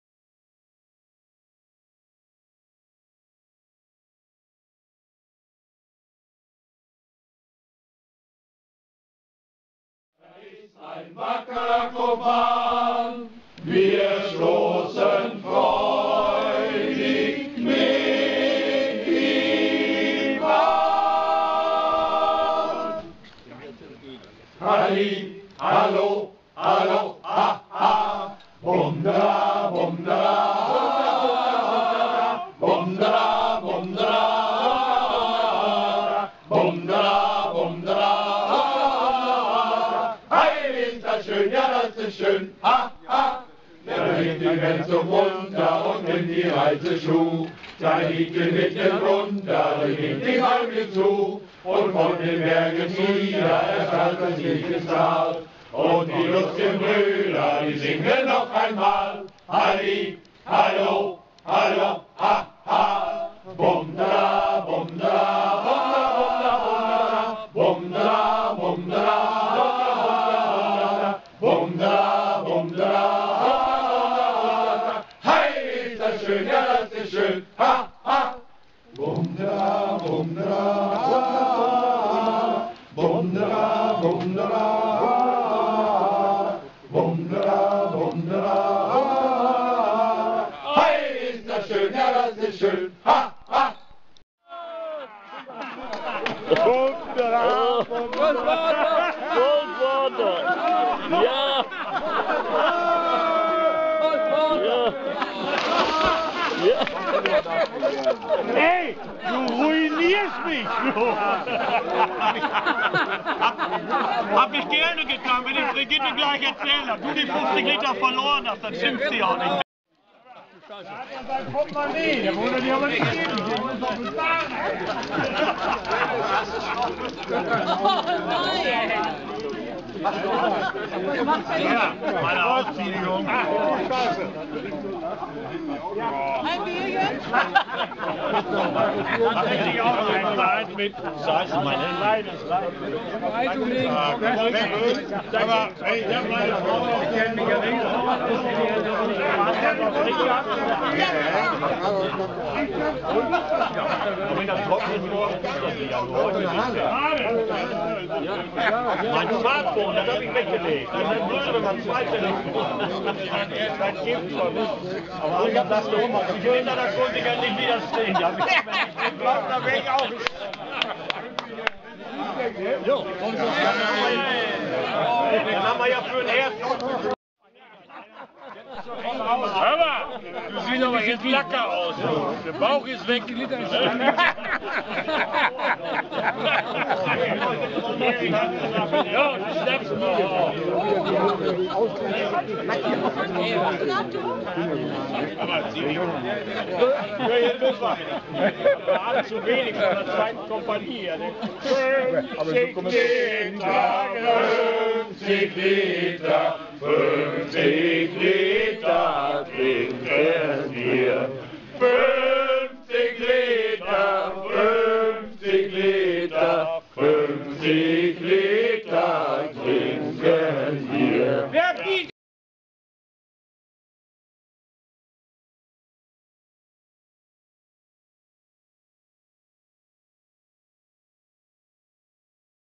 50 Liter trinken wir
Bei einem Gartenfest wurde eine Wette um 50 Liter Bier abgeschlossen mit der Auflage, in voller Montur ins kalte Schwimmbad zu tauchen.